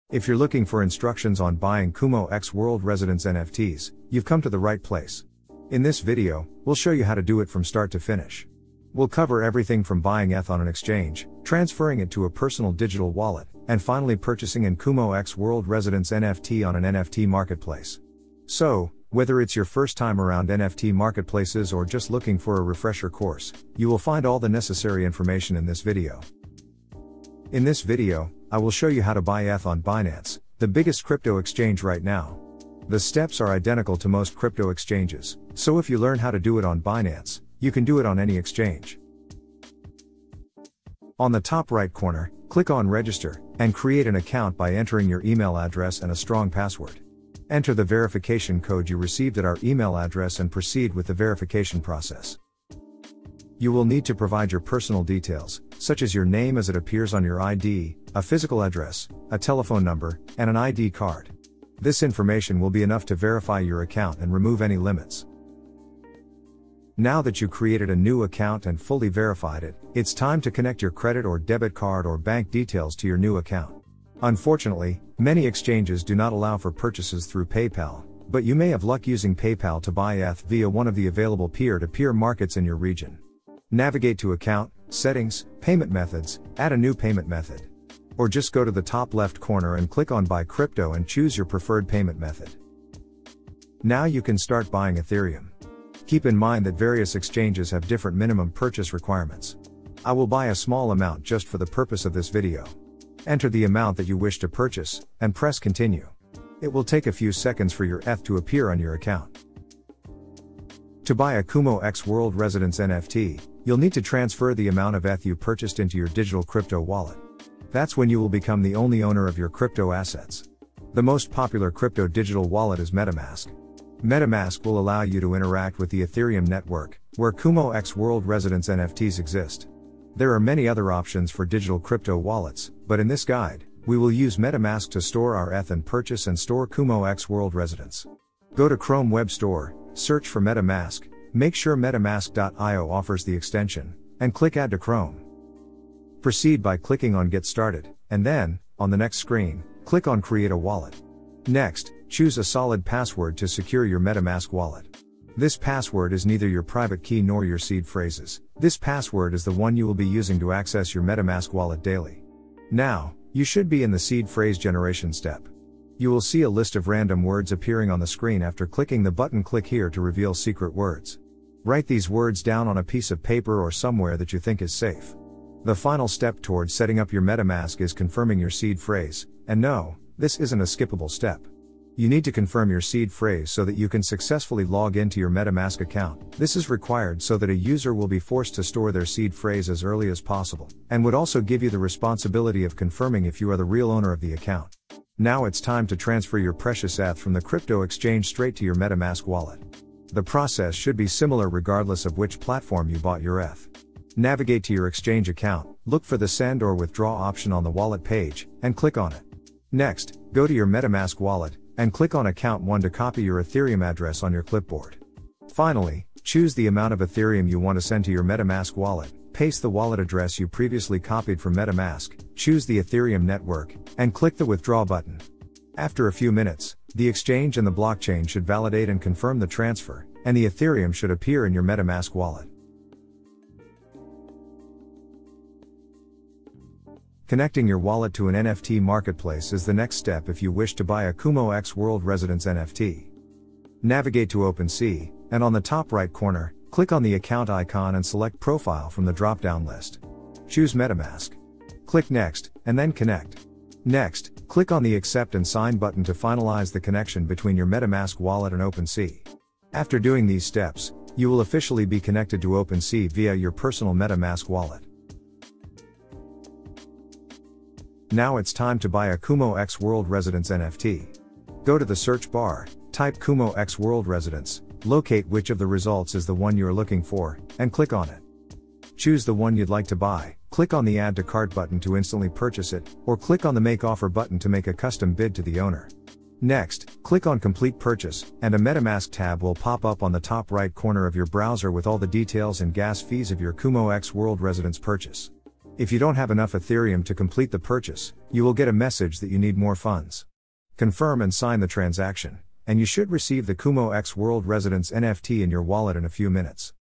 How and Where To Buy Kumo x World Residents NFTs - Detailed Guide
Want to know how and where to buy Kumo x World Residents NFTs? It's the most crucial part of getting your first Kumo x World Residents NFT.
Choosing the right exchange, wallet, and marketplace to buy a Kumo x World Residents NFT is crucial and can possibly save you a lot of time and money if done right.

Want us to tell you our favorite way of buying Kumo x World Residents? Well, let's dive right in.

TIMELINE
0:00 What you will need to do
0:33 Turn Cash into ETH
1:49 Move your ETH to a Wallet
3:52 Connect wallet to a marketplace
4:22 How to buy Kumo x World Residents NFTs

Step one, buying some Ethereum with cash. I see a lot of beginners make mistakes in this phase. Maybe they'll choose an exchange without doing any research and end up paying high fees or even being unable to use the exchange because it's not supported in their country. Since you will need a good amount of ETH to buy Kumo x World Residents, it might be a good
idea to use our website to see the best exchanges with the lowest fees.

Step two, moving the ETH you bought to your own crypto wallet. Ready to be the actual owner of your crypto? That's what we always recommend people to do. Get their crypto in their own wallets to have
full control. This is a mandatory step to buy a Kumo x World Residents NFT.

Step three, connecting your crypto wallet to an NFT marketplace. The last crucial step before actually buying your favorite Kumo x World Residents NFT is to connect your wallet to an NFT marketplace which in this case
will be OpenSea.

Step four, buying Kumo x World Residents NFTs. This is the final step, where you will actually purchase the Kumo x World Residents NFT that you want and safely store in your Metamask wallet.